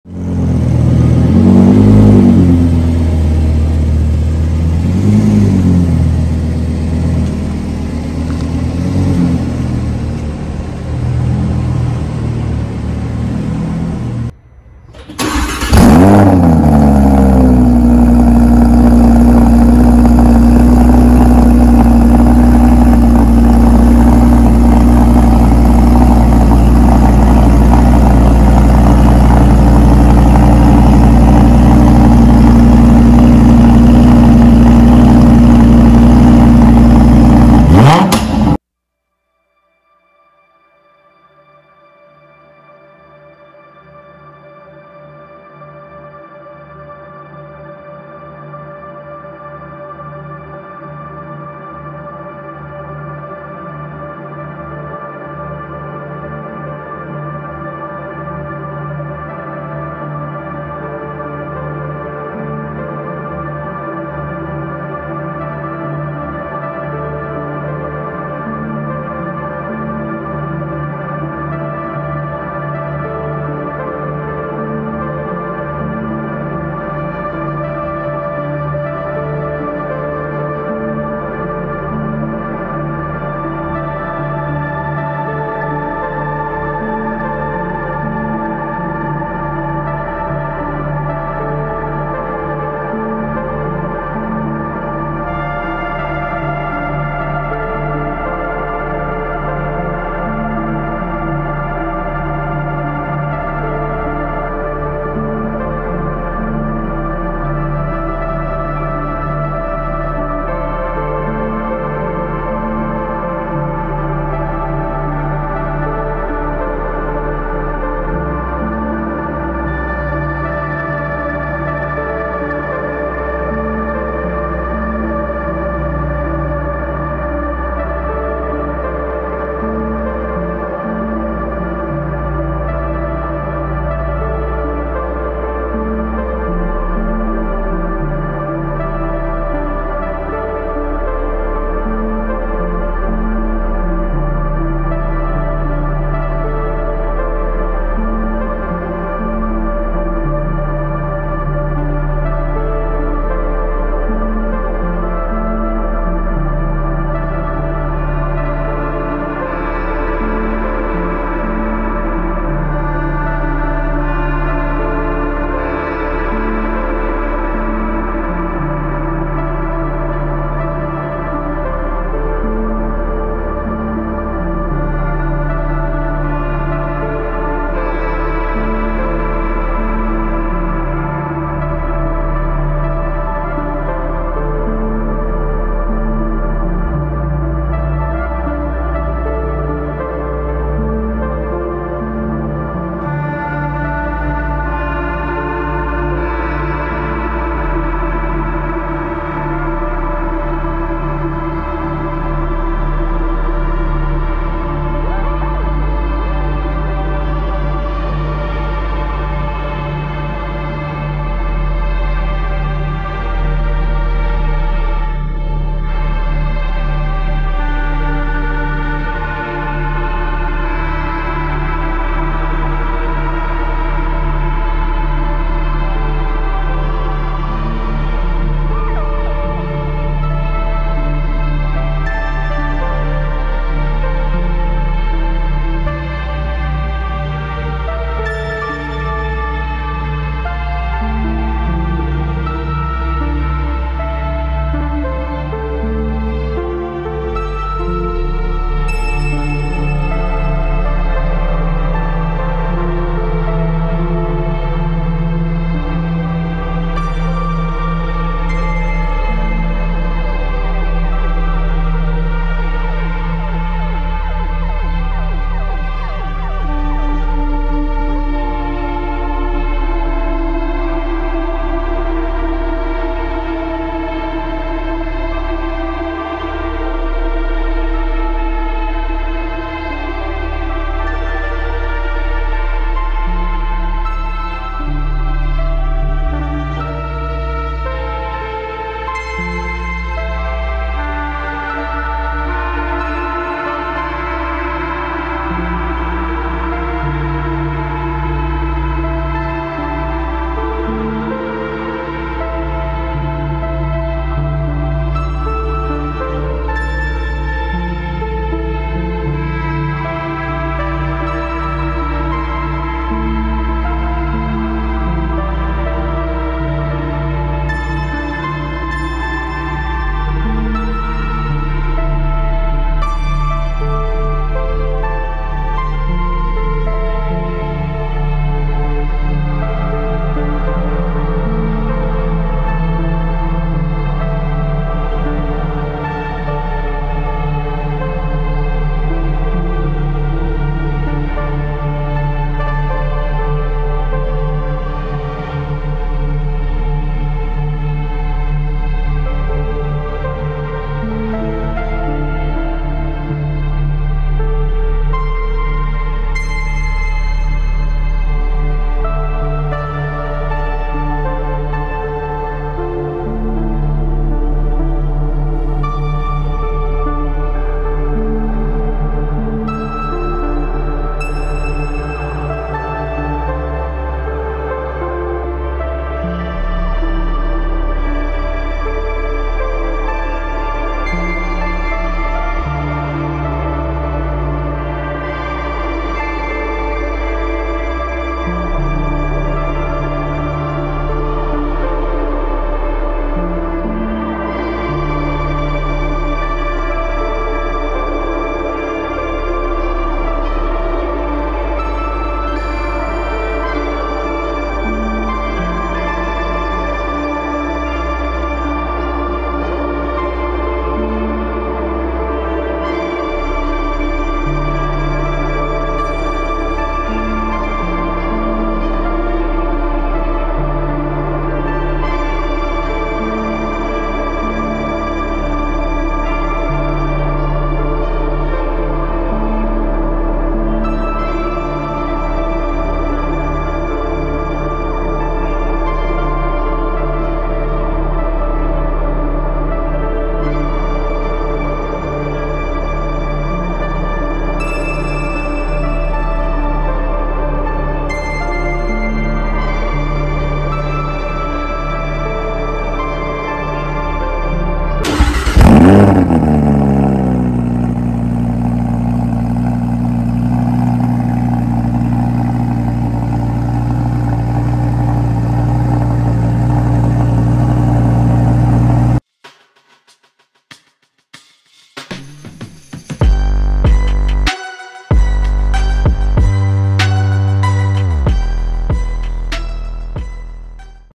2023 AUDI R8 | AUDI R8 V10 FSI 😍
2023 AUDI R8 | AUDI R8 V10 FSI

5.2L V-10 Engine
7-spd auto-shift man w/OD Transmission
562 @ 4,475 rpm Horsepower
406 @ 6,300 rpm Torque
Rear-wheel Drive type
ABS and driveline Traction control
20" machined w/painted accents forged aluminum Wheels
MMI Navigation plus with voice activation Navigation system
Audi parking system plus Parking assist